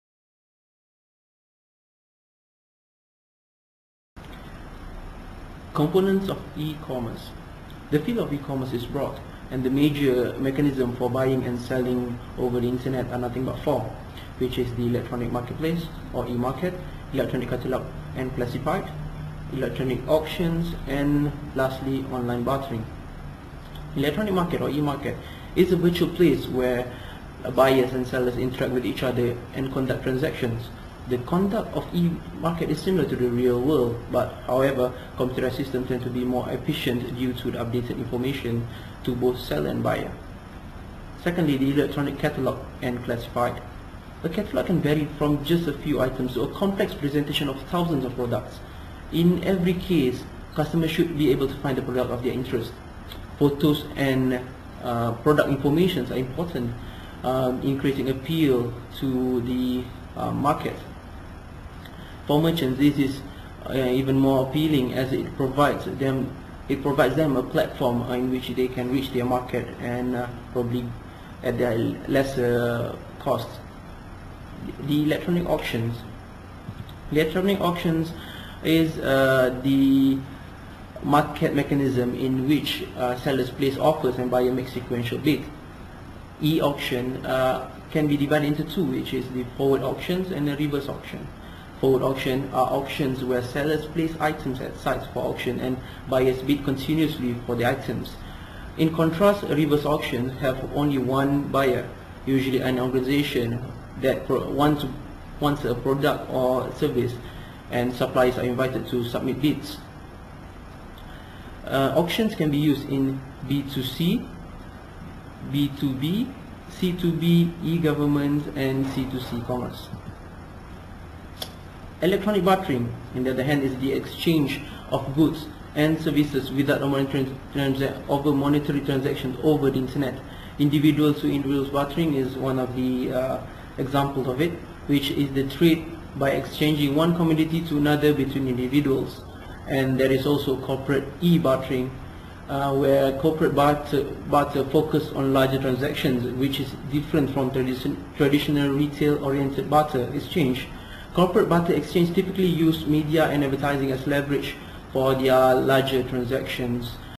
Components of eCommerce.wmv
A video presentation for MIS 740 - Managerial Information System on the topic of "Components of e-Commerce" for Assoc. Prof Dr. Norzaidi Mohd Daud (FBM, UiTM). Presentor: Mohd Adi Mohd Kamal (Post-grad student, FBM, UiTM).